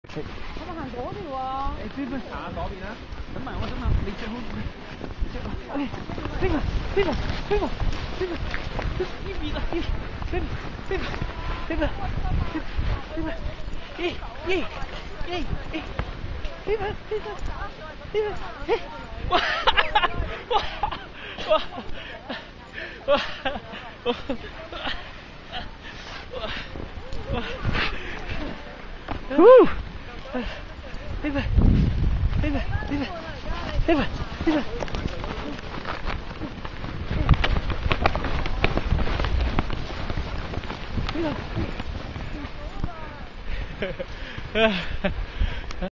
Tim snow shoeing with Paper Husky in the deep think snow! Very funky...Mt. Seymour
Videographer: ?
Model: Tim Fan and Paper 2 yrs old Husky
Location and Event: Esquare snow shoeing adventure, Mt. Seymour, North Vancouver